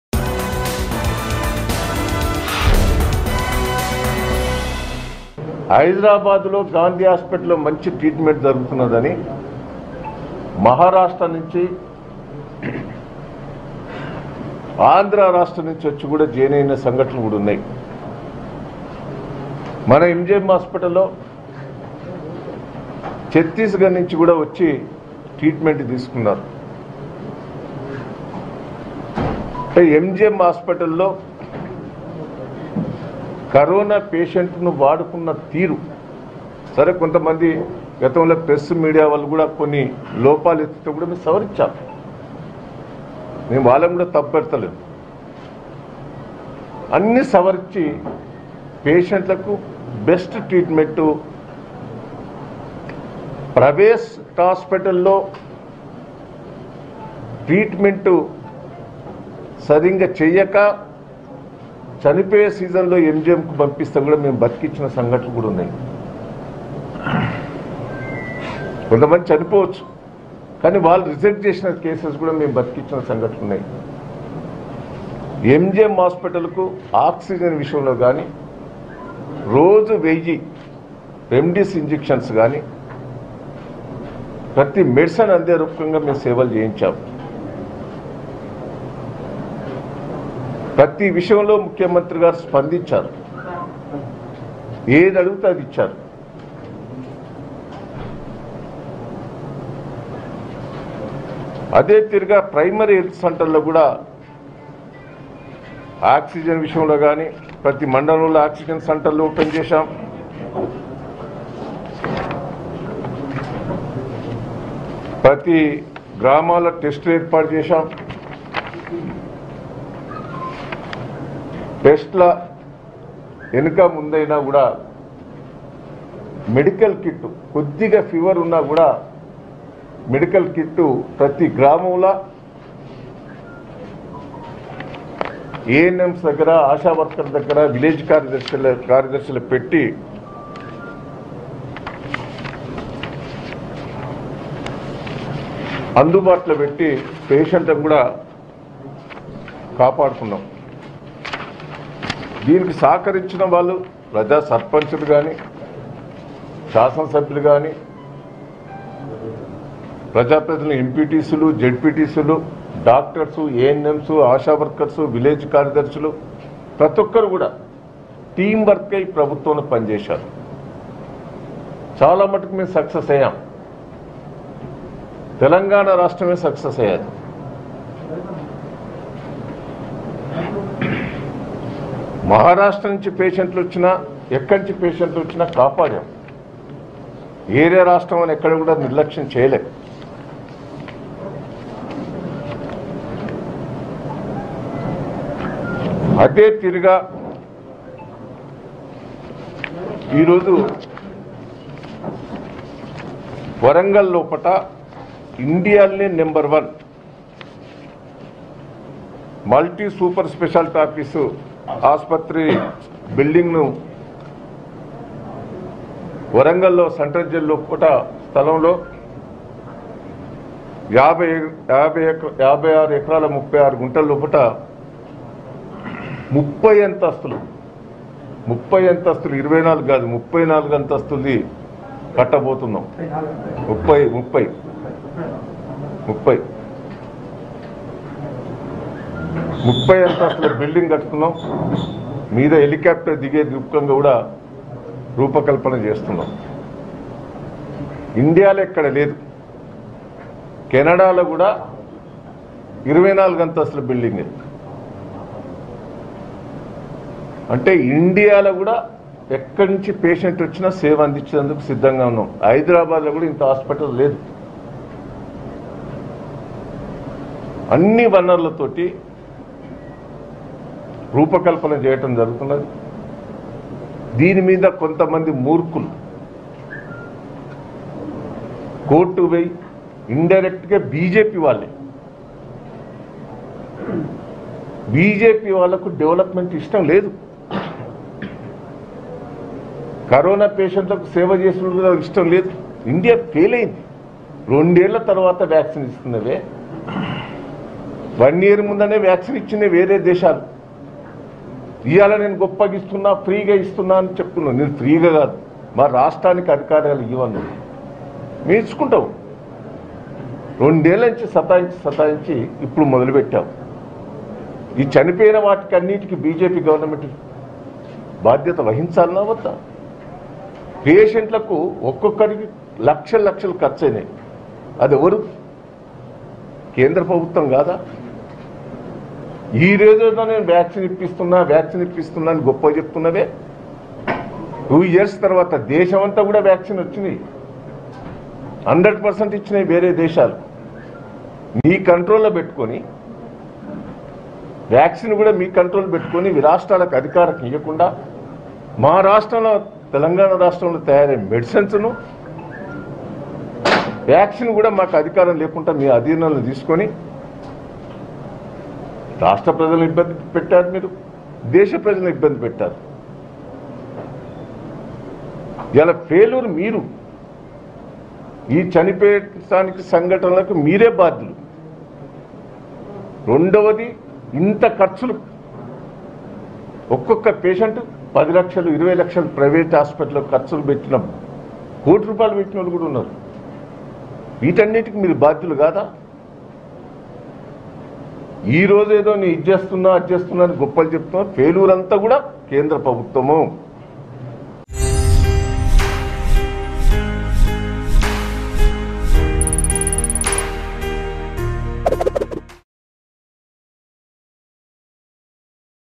Errabelli Dayakar Rao Speaks About Launching Super Speciality Hospital In Warangal | Mango News
Errabelli Dayakar Rao Speaks About Launching Super Speciality Hospital In Warangal.